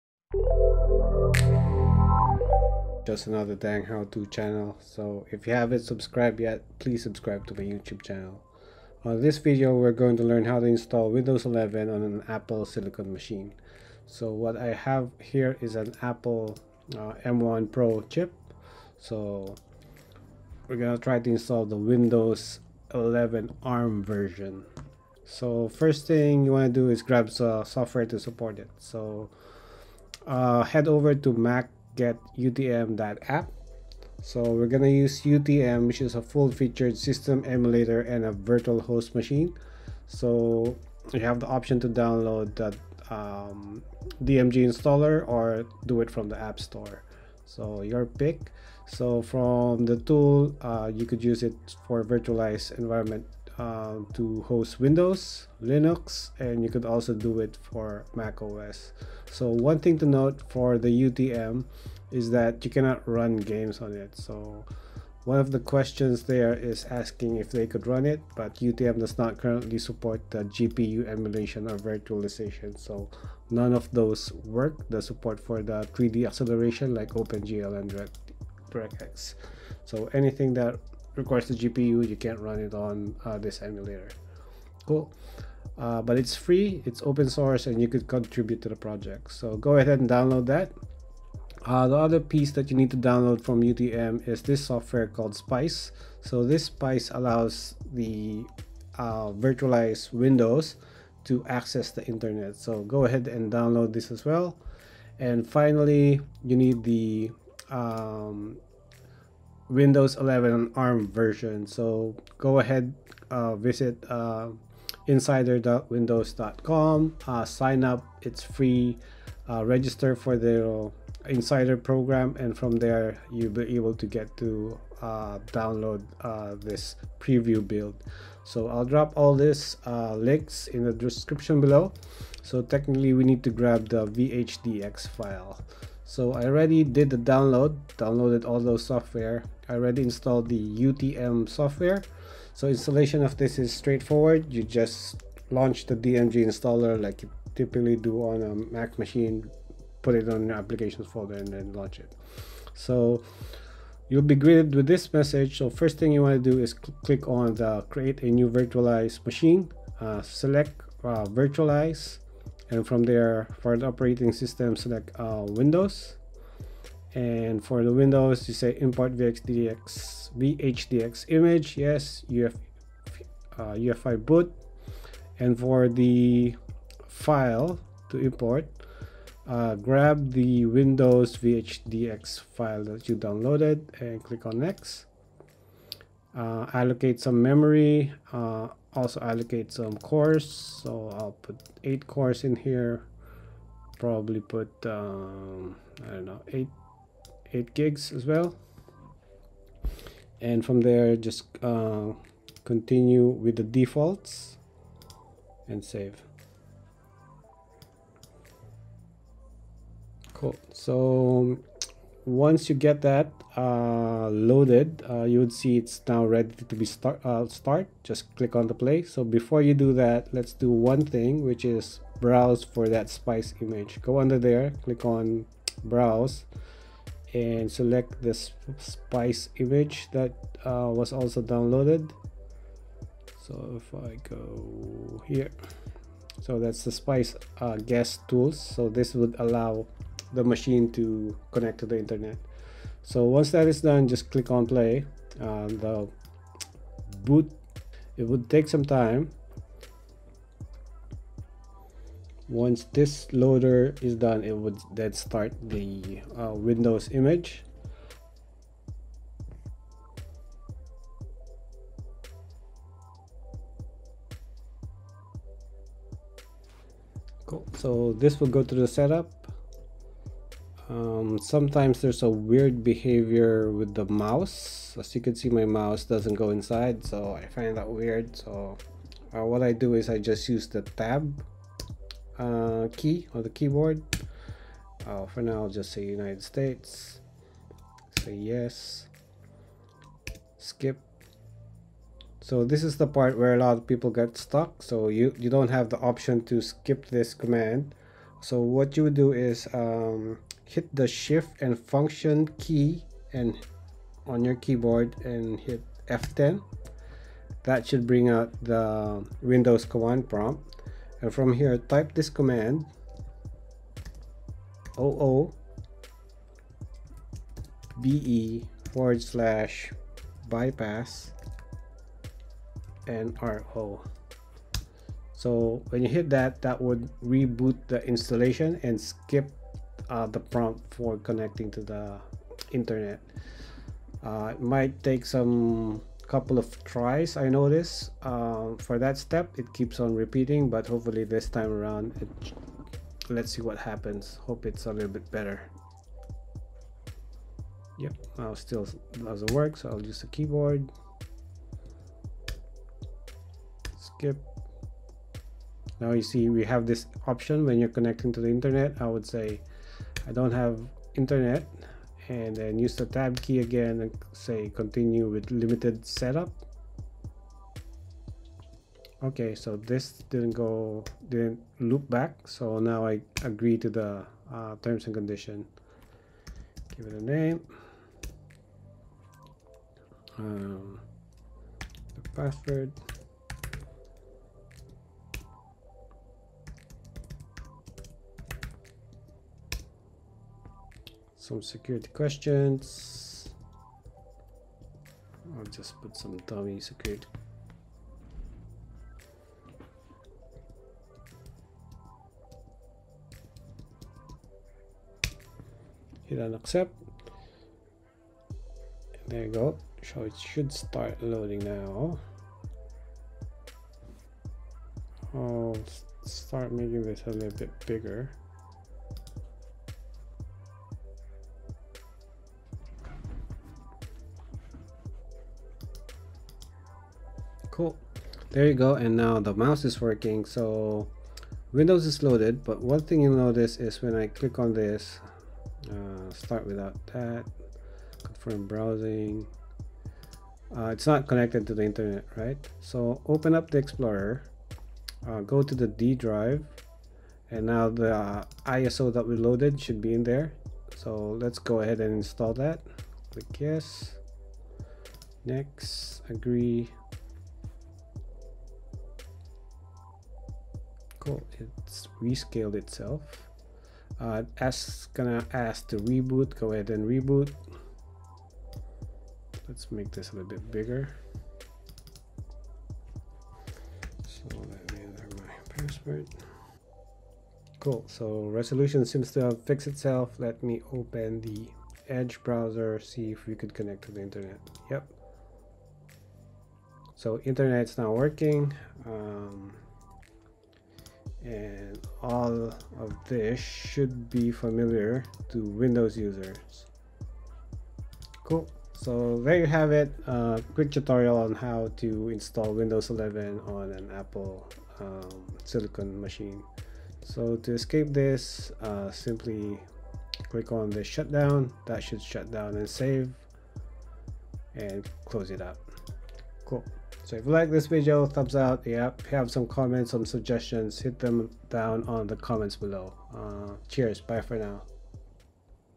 How To Install Windows 11 ARM on an Apple Silicon Machine
On this video we are going to learn how to install Windows 11 on an Apple Silicon Machine.

To bypass the connect to internet setup.
Hit
Shift+fn+F10

Enter
OOBE\BYPASSNRO

Workstation Setup
Macbook Pro 16 inch M1 Pro

Samsung 28-Inch 4K UHD Led-Lit Monitor

Apple Magic Keyboard with Touch ID and Numeric Keypad